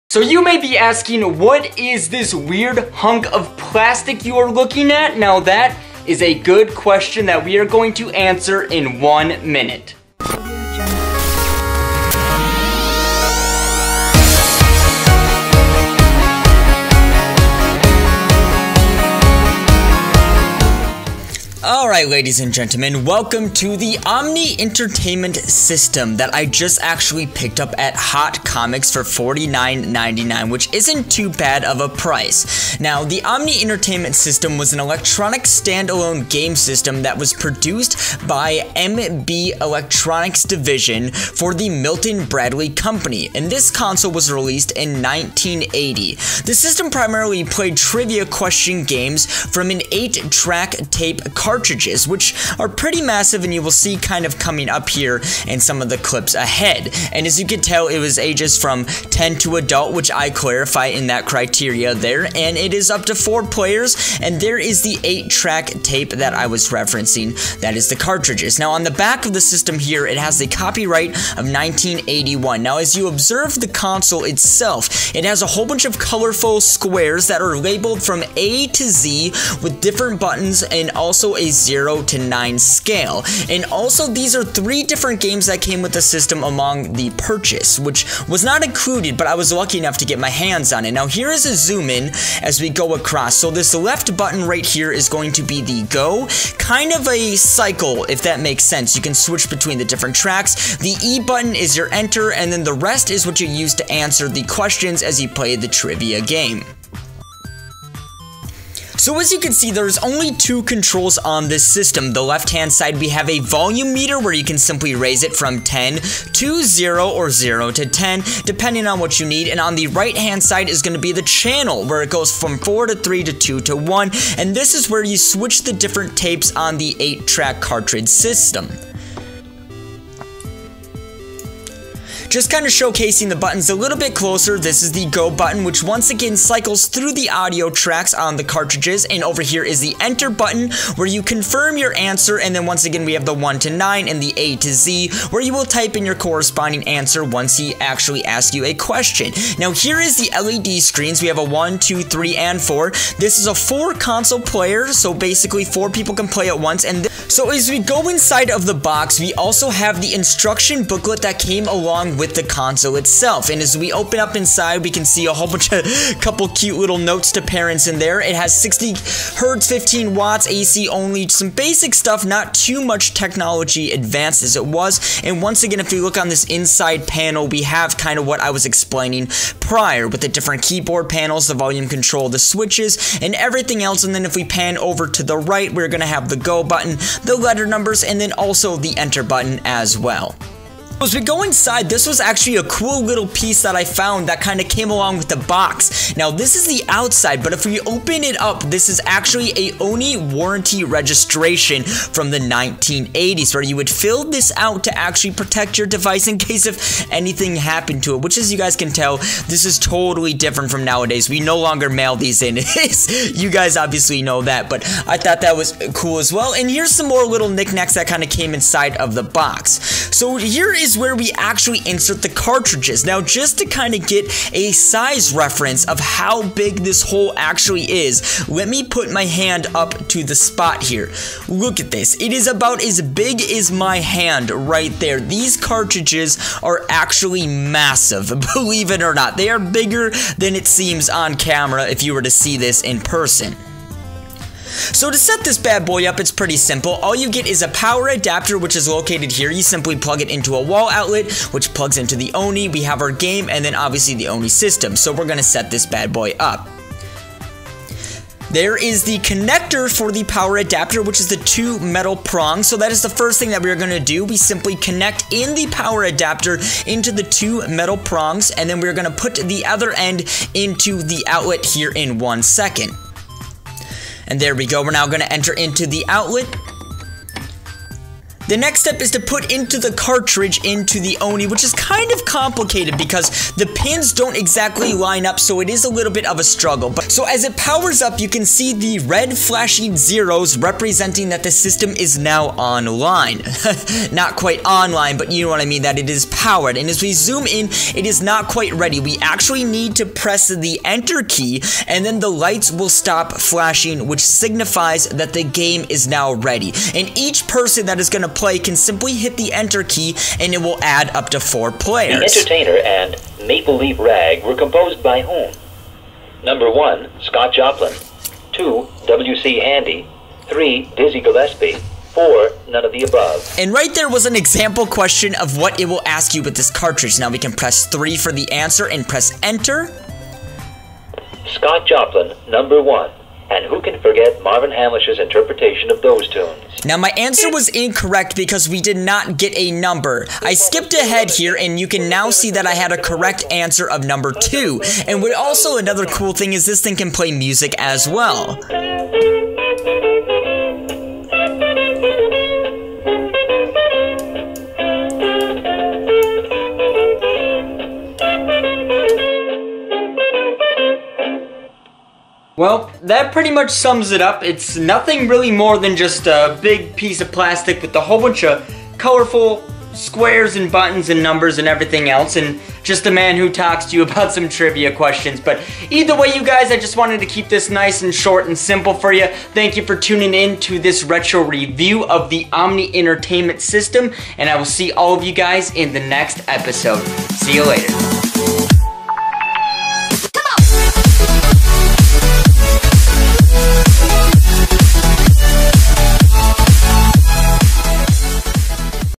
Omni Entertainment System | Retro Review - Episode 1
Welcome to Retro Review! A new series i'm starting on the channel to showcase my Retro Collection. Today is the Omni Entertainment System, as we unbox and preview this old gem.